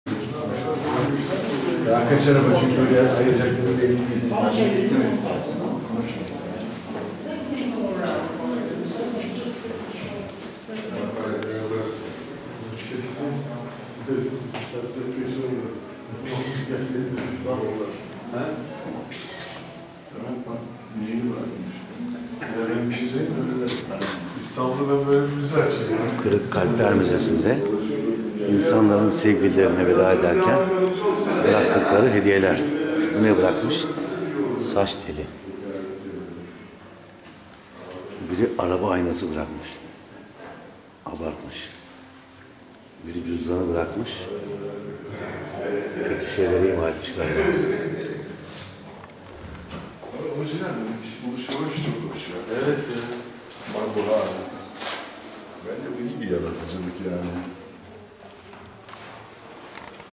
Kırık Kalpler Müzesi Zagreb... Geçmişi silen silgi!
Zagreb'te dünyanın en ilginç müzelerinden biri... Kalbi kırık olanlar birşeyler bırakıp gidiyor.